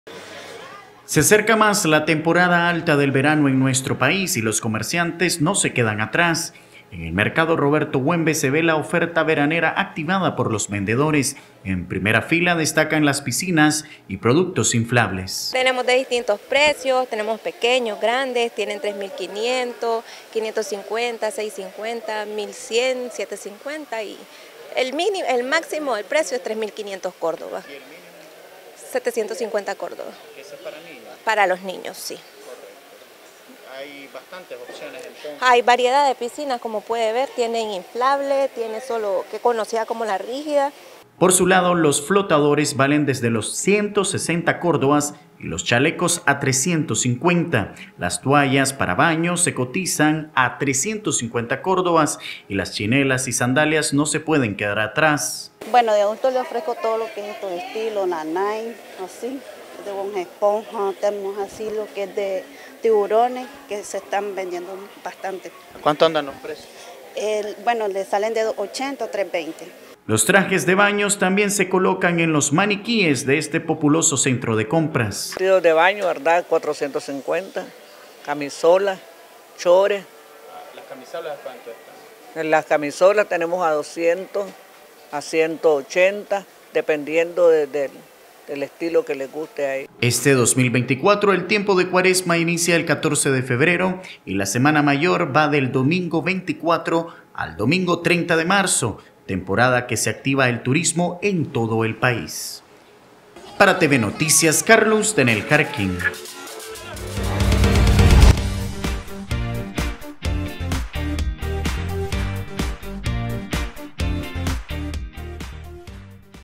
Comerciantes empiezan a activar productos veraneros en los mercados
Desde ya algunos comerciantes del Mercado Roberto Huembes activan su oferta de articulos veraneros, cuya temporada alta se acerca cada vez más; conozcamos los productos que más demanda la población en las próximas semanas.